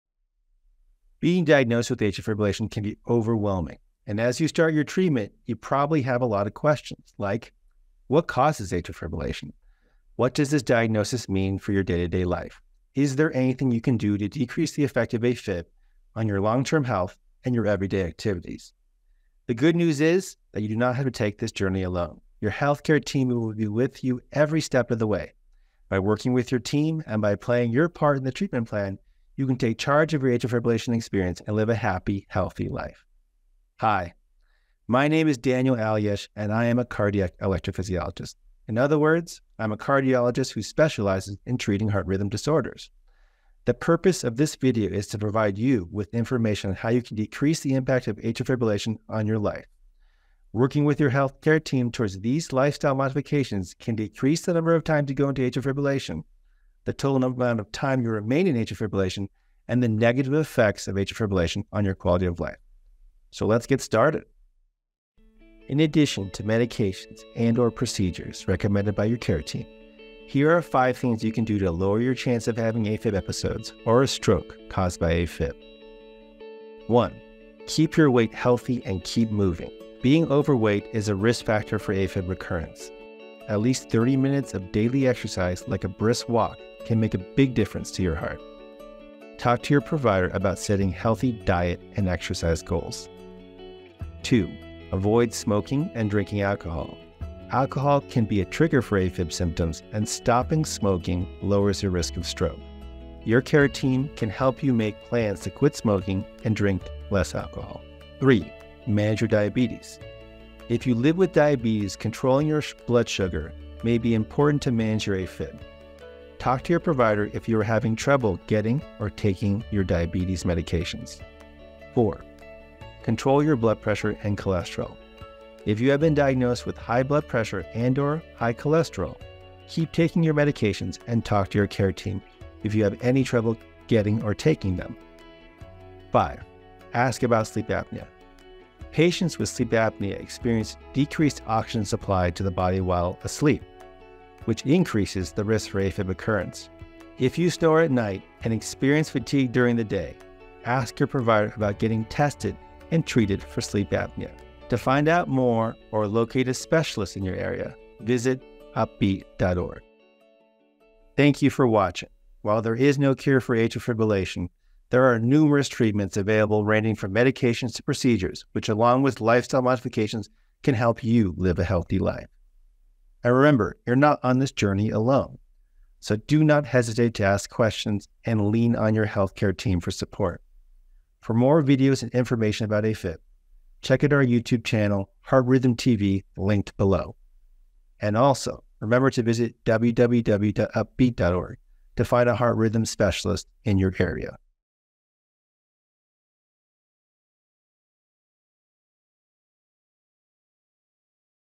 EP Explained: Episode 2 - AFib Lifestyle Modification (English)
In this second installment of "EP Explained", HRStv host Daniel M. Alyesh, MD, FHRS, covers AFib Lifestyle Modification: including what causes AFib, what does the diagnosis mean, and how it will impact your daily life.

For more patient-friendly resources, visit UpBeat.org.